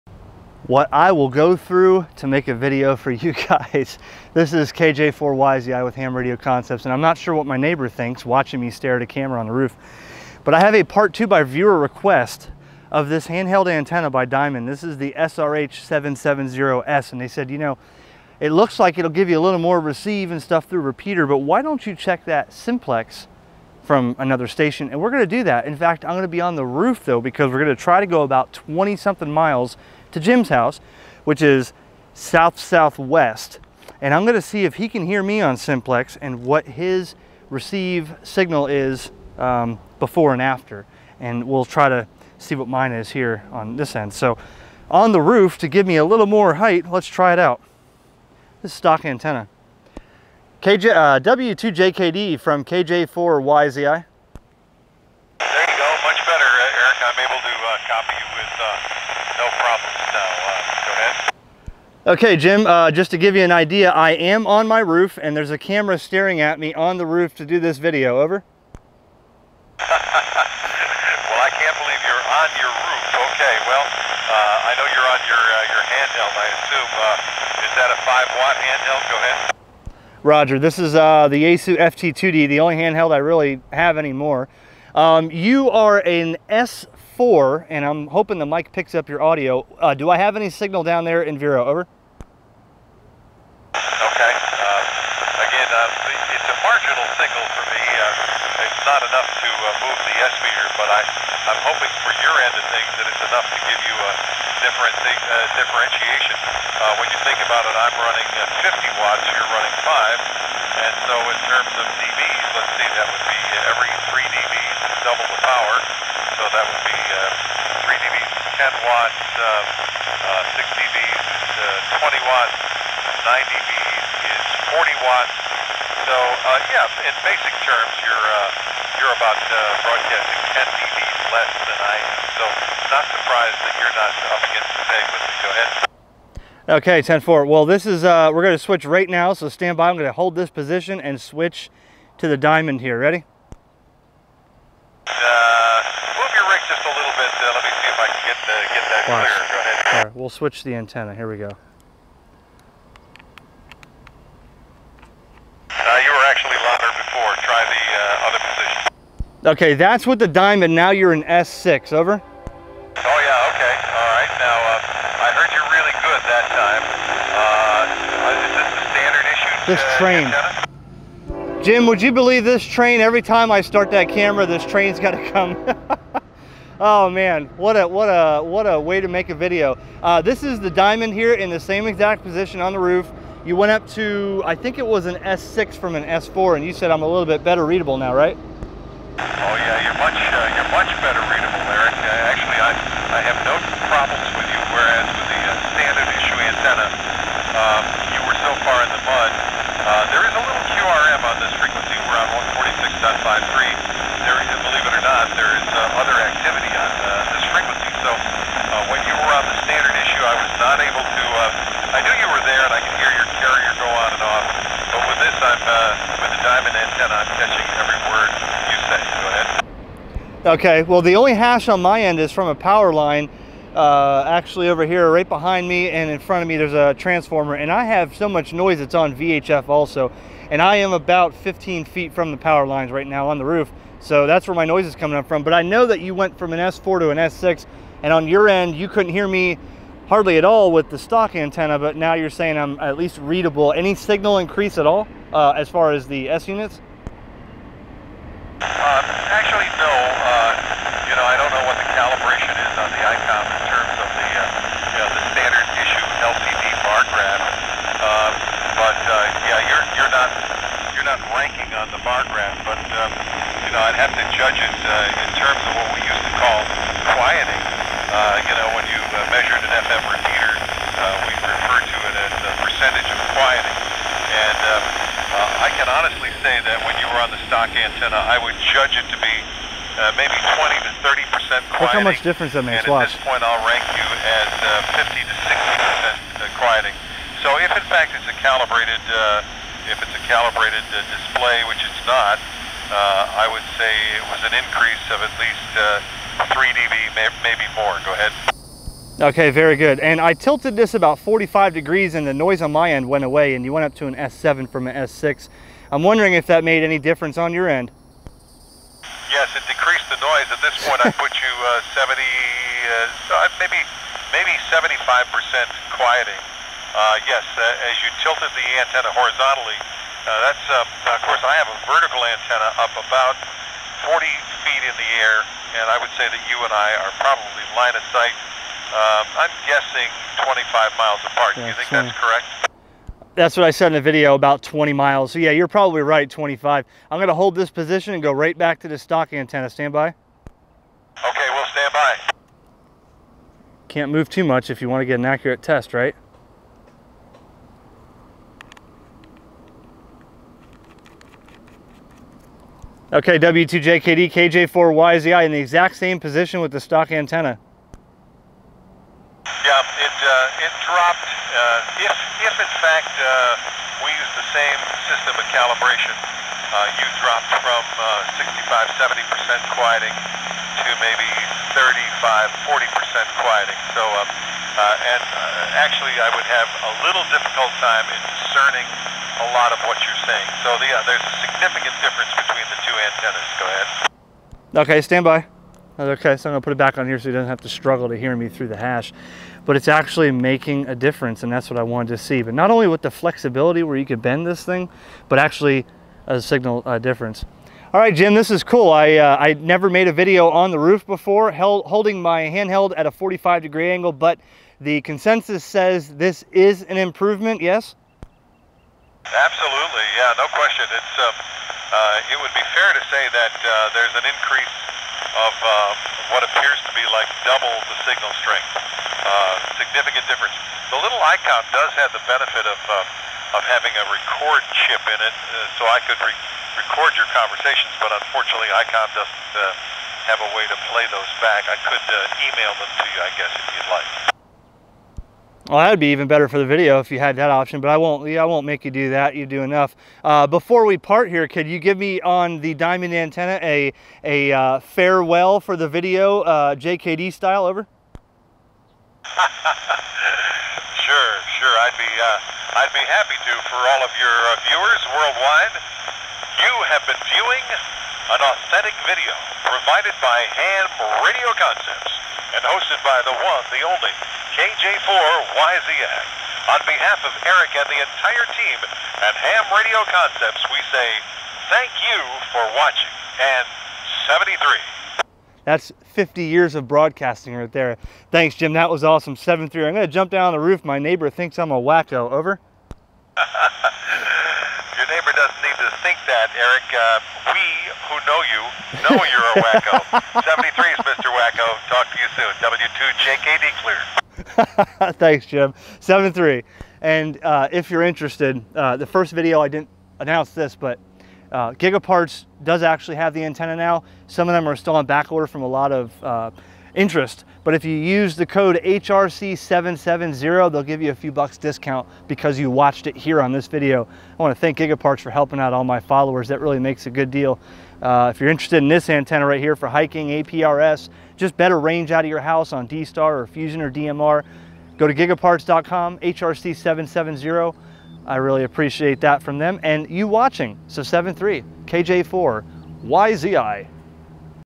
Ham Radio HT Antenna Range Test, Diamond SRH770S
Another video on this new antenna from Diamond, by viewer request, to see how much difference it makes on simplex with a distant station. Diamond SRH770S dual band flexible antenna, 3/8 wave on 2m, 3/4 wave on 70cm.